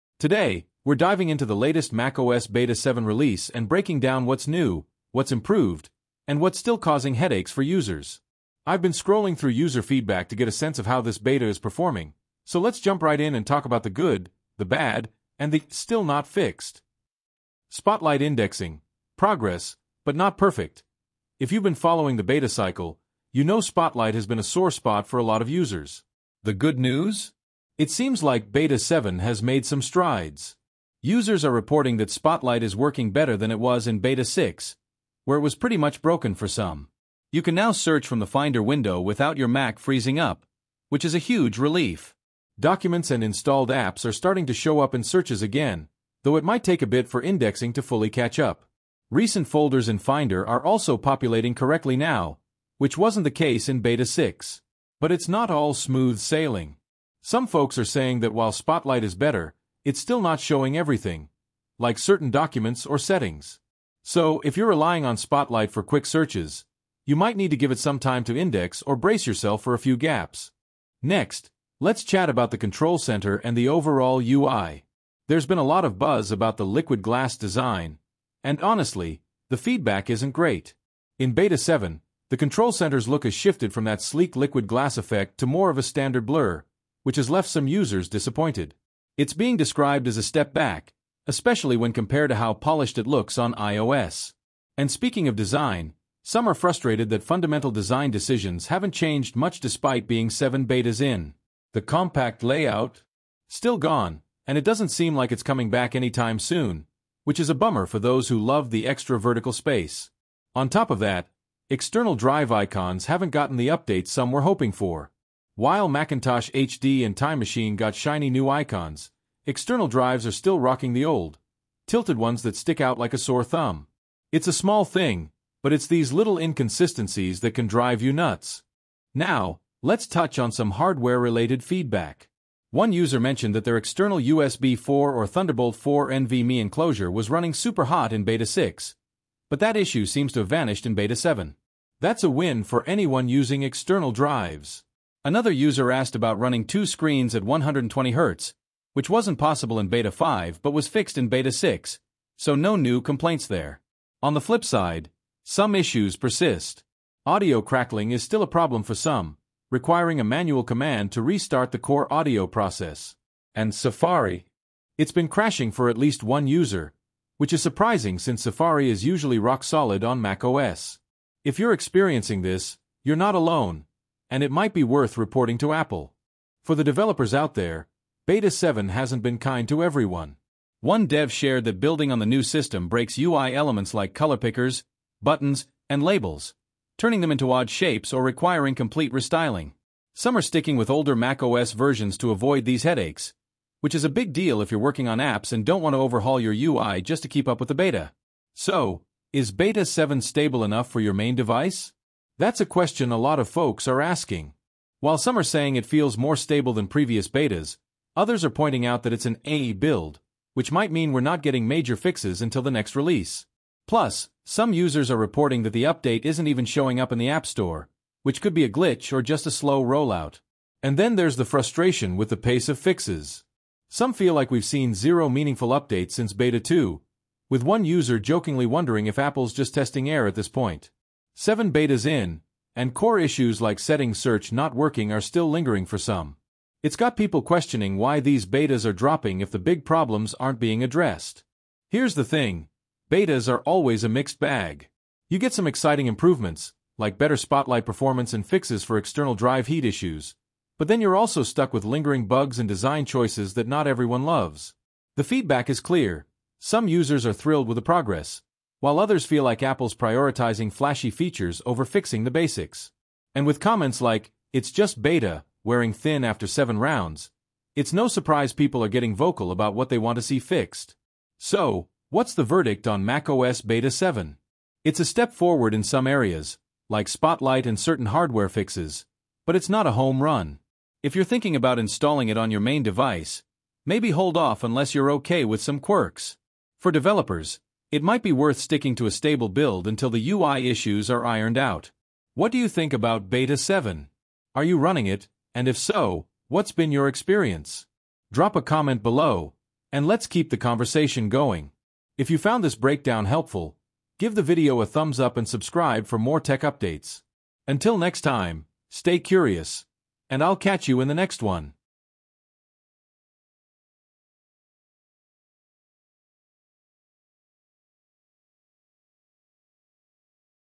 The NEW macOS 26 Tahoe Beta 7
The latest macos 26 beta 7 is here, and this video gives a quick overview. Learn about the newest features and improvements, including the apple intelligence updates and changes to spotlight search tips. This video also goes over the macos troubleshooting steps and how to fix mac spotlight issues.